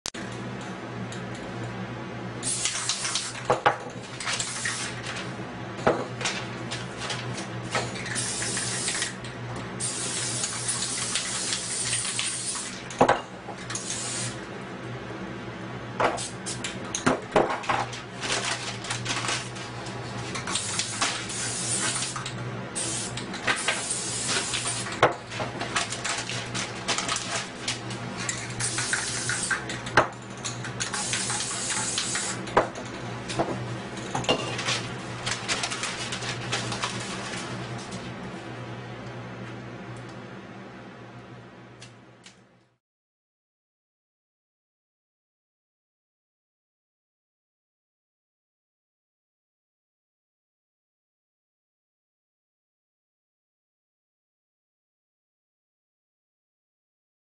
Super Fast Spray Paint Artist
This artist can achieve textures within in seconds. Very impressive.
Super Fast Spray Paint Space Painter
Using spray paint and magazine paper this guy creates a cool space scene.
get a 1 minute painting here